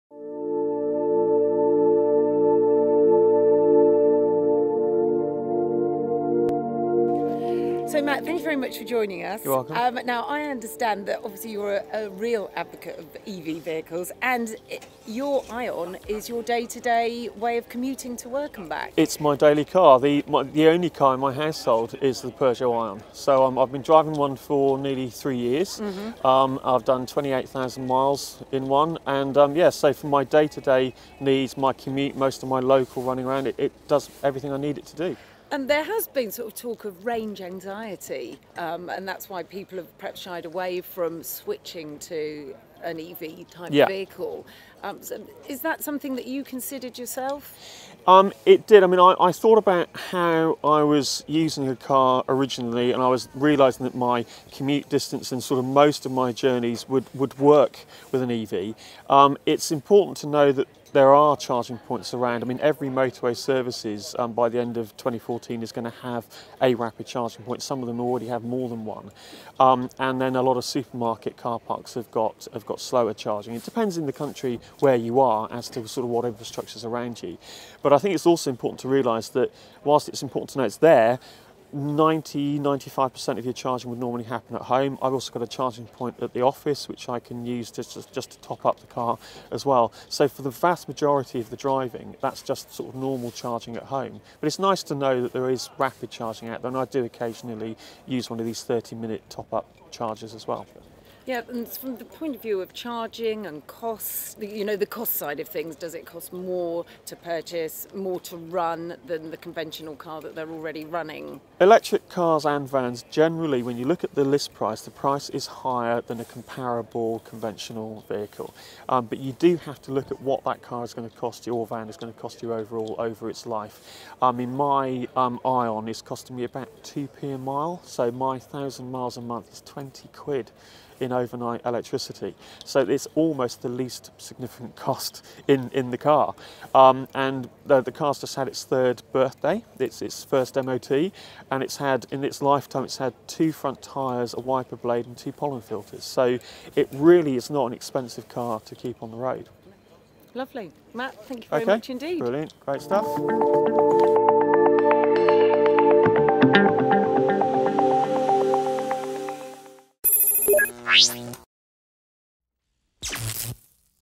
Peugeot iOn | An interview with Matt Trevaskis - EV Specialist
Peugeot's very own Matt Trevaskis talks to the IET about the Peugeot iOn